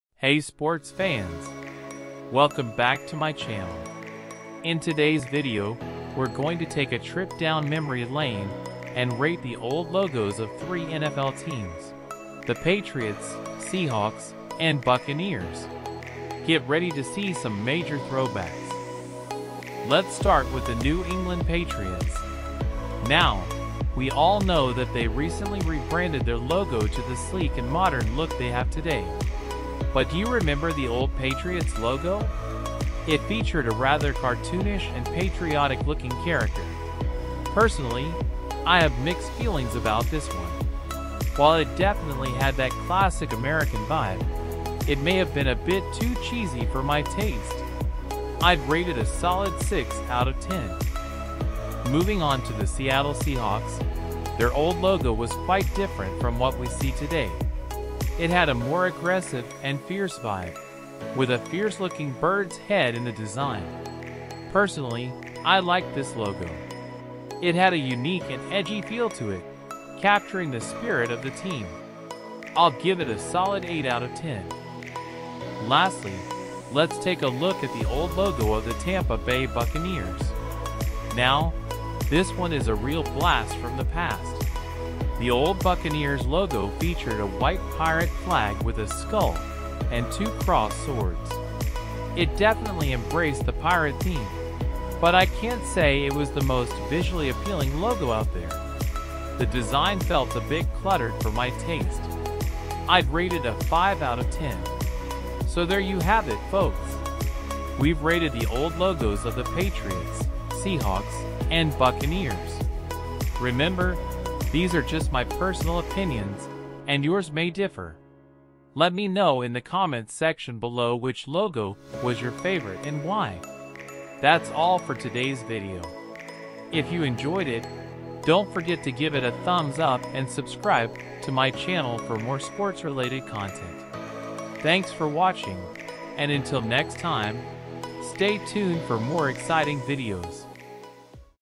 Rating NFL Logos pt.2
0:00 - Interdiction
0:19 - Patriots
0:51 - Seahawks
1:15 - Buccaneers
1:45 - Outro